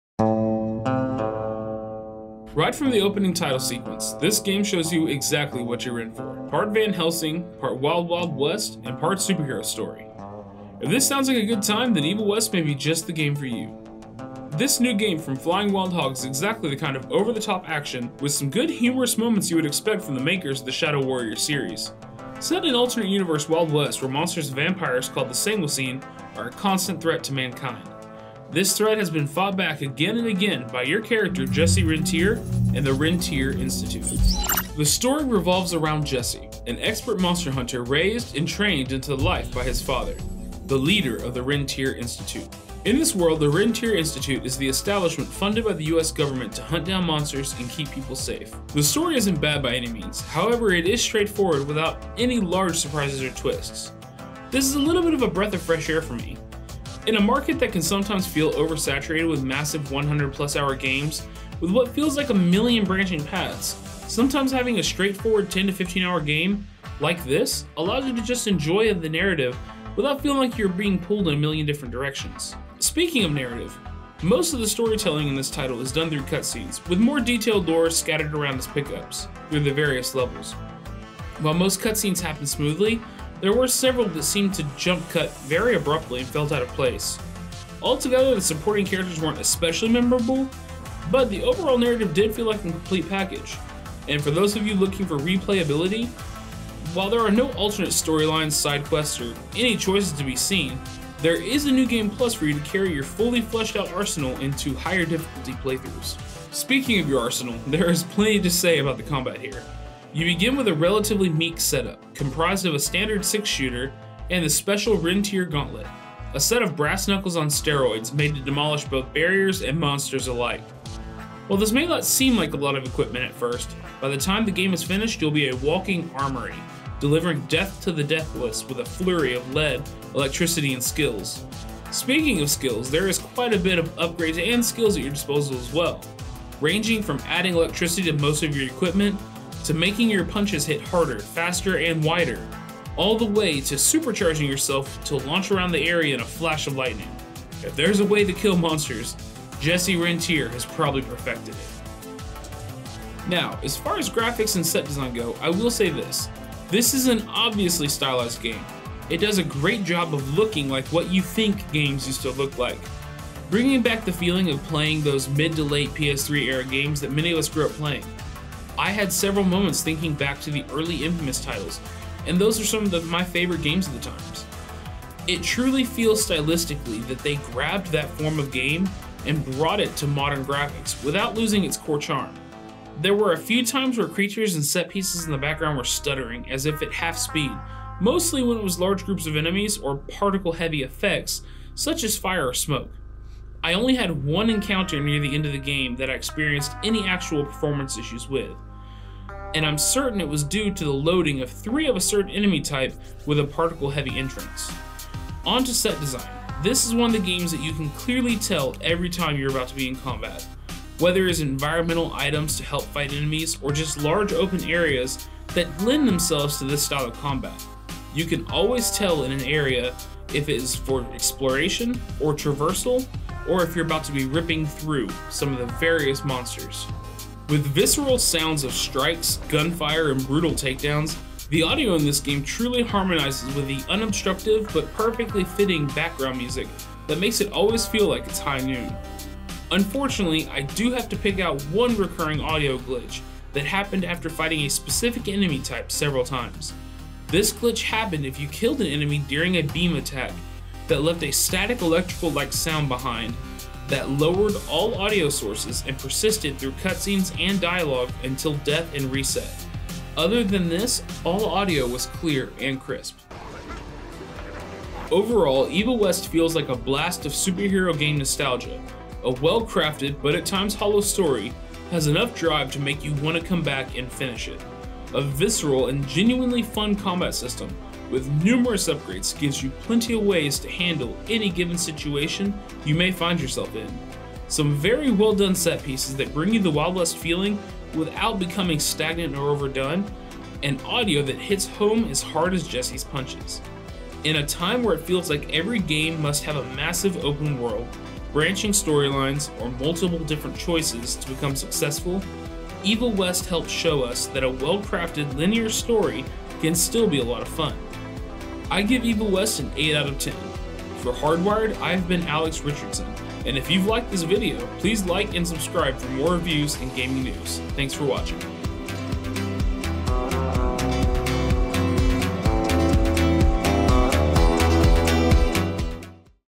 EVIL WEST Reviewed: Cowboys, Monsters and Synth-wave (Review)
Right from the opening title sequence this game shows you exactly what you’re in for. Part Van Helsing, part wild wild west, and part superhero story. If this sounds like a good time, then Evil West may just be the game for you. This new game from Flying Wild Hog is exactly the kind of over-the-top action with some good humorous moments you would expect from the makers of the Shadow Warrior series. Set in an alternate universe wild west where monsters and vampires called the sangusine are a constant threat to mankind. This threat has been fought back again and again by your character Jesse Rentier and the Rentier Institute. Is Evil West an instant cowboy classic, or a remnant of bygone era?

HERE IS OUR HONEST REVIEW BY OUR OWN ALEX RICHARDSON